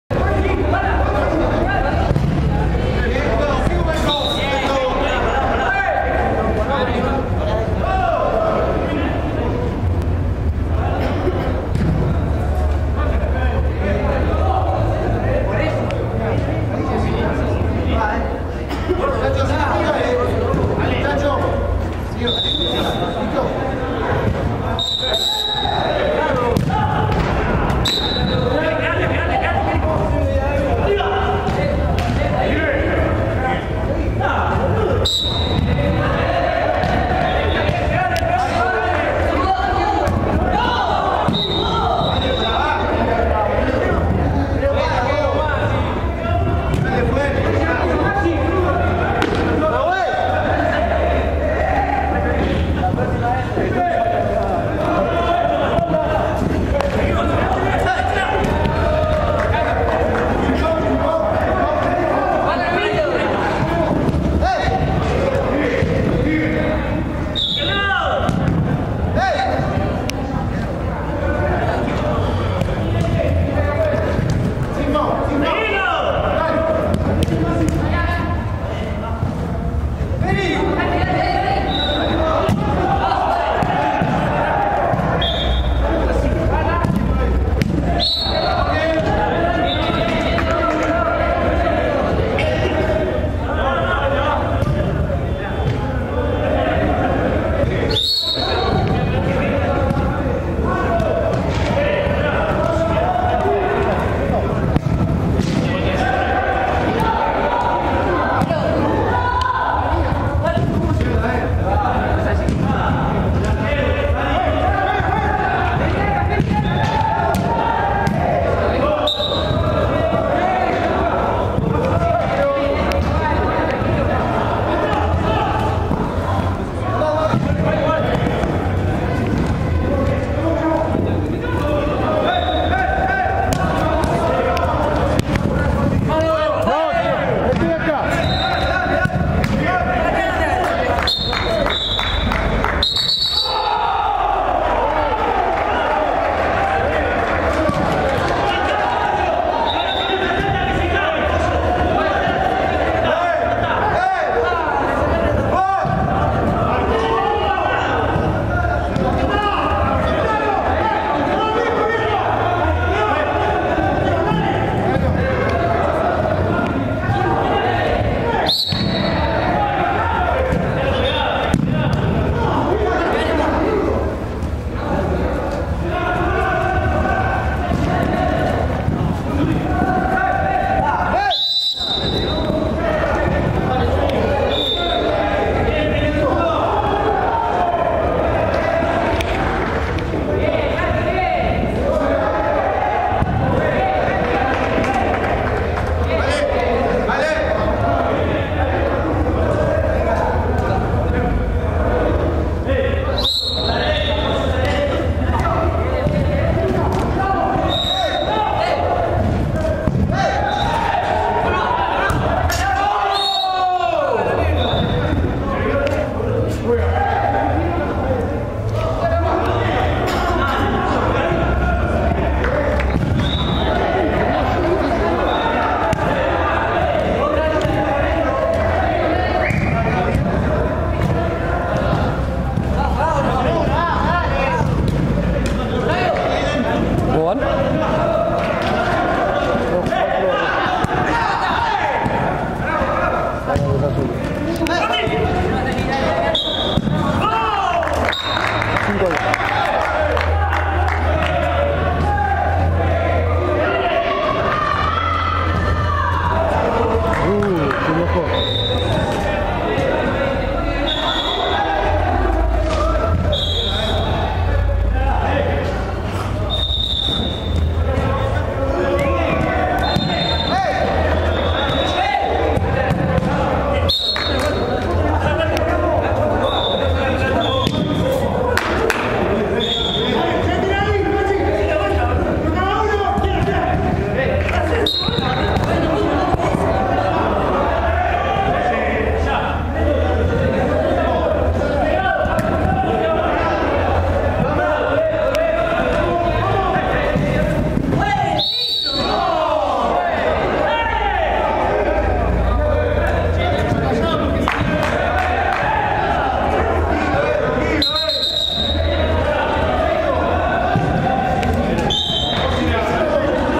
Torneo Carrizo - Octavos de Final: Pero que viva el fútbol Pisculichi vs Iriarte Francescoli Club
Torneo de Fútbol 5 para Hinchas de River

Anotate!